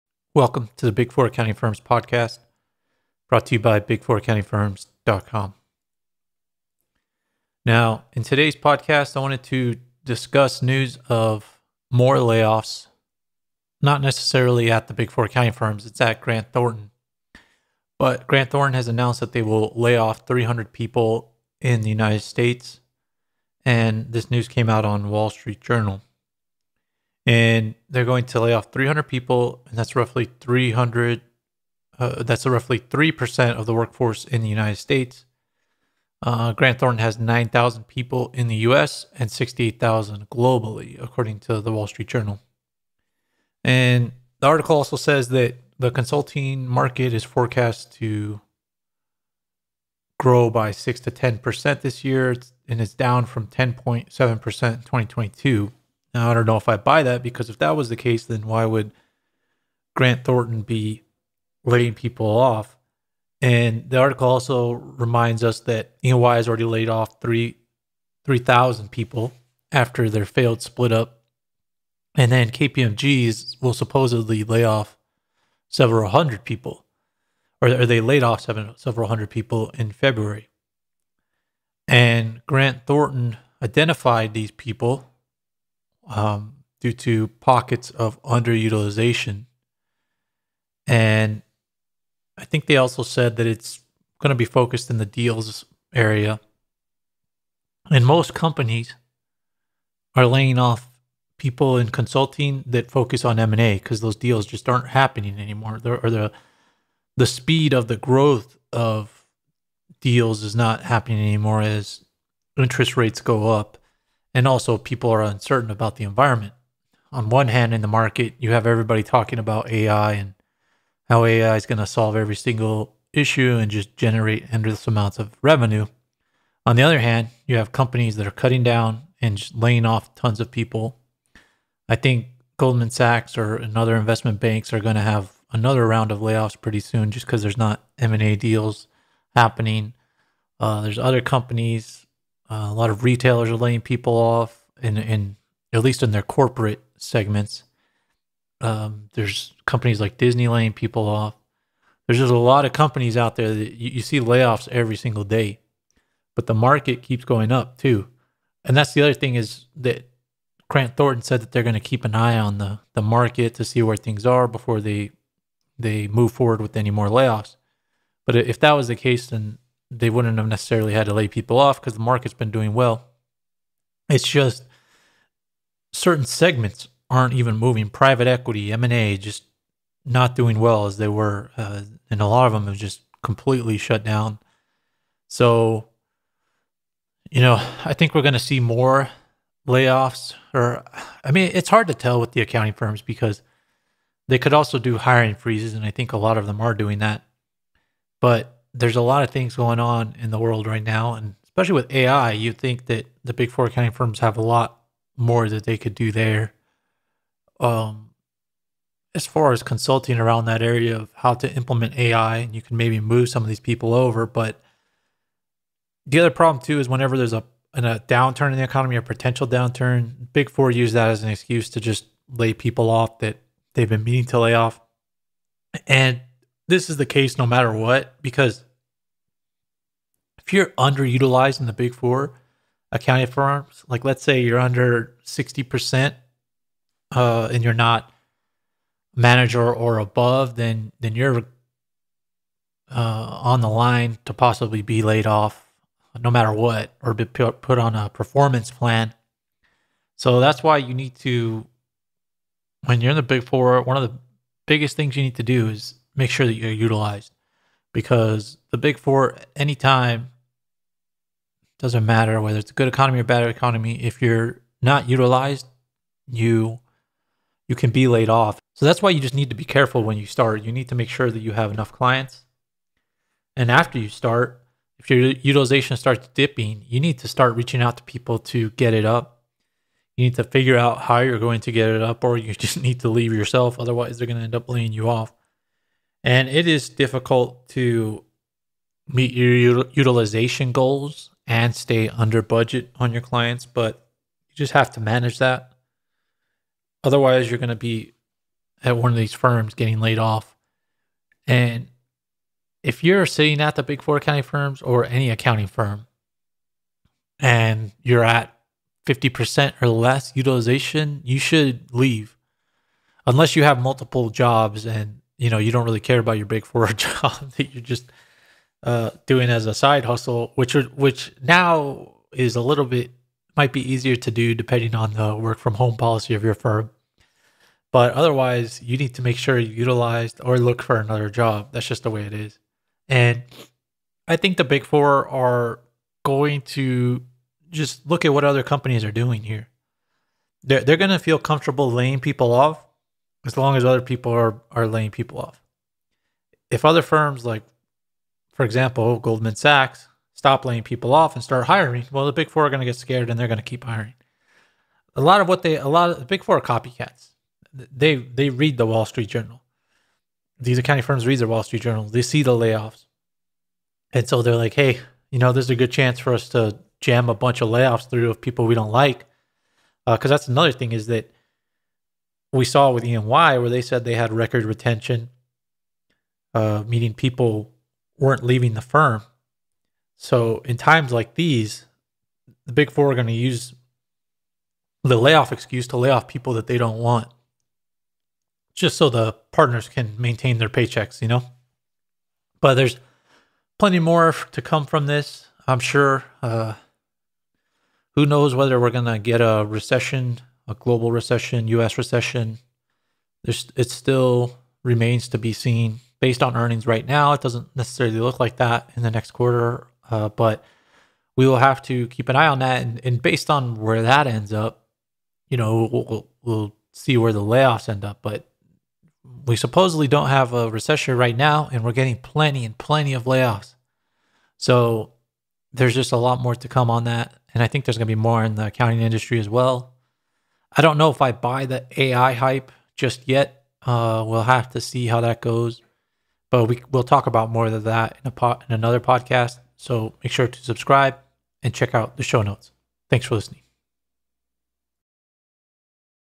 Grant Thornton Lays Off 300 Employees
In today's podcast we discuss news that Grant Thornton is laying off employees